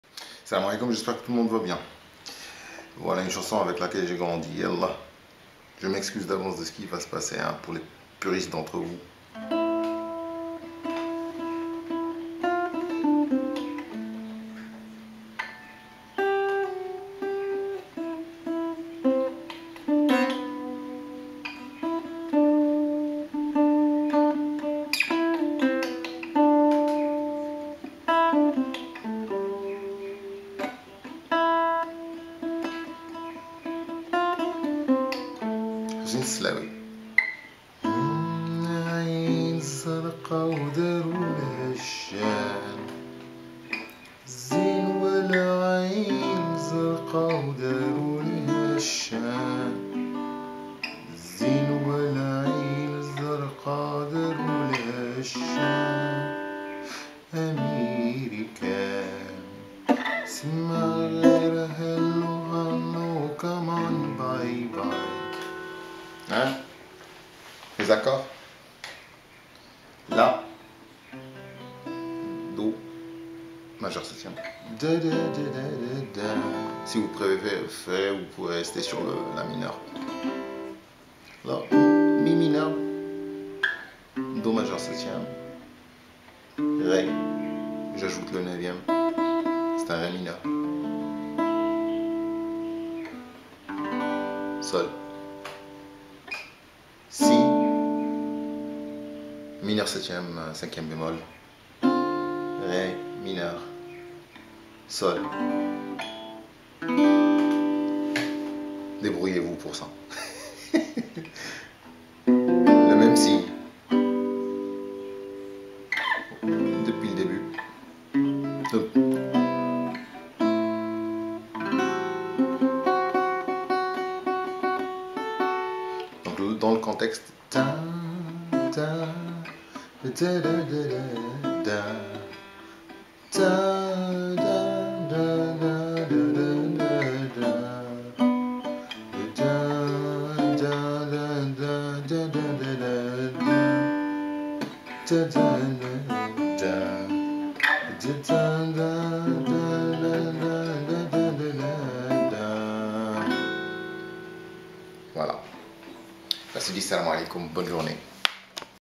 Lhaj Houssine Slaoui (رحمة لله عليه) vous salut
Une chanson que j'entendais ma maman chantoner et avec laquelle j'ai grandi. Voila Je m'excuse d'avance pour le charcutage. Ghir ma galia rassi fhad sbah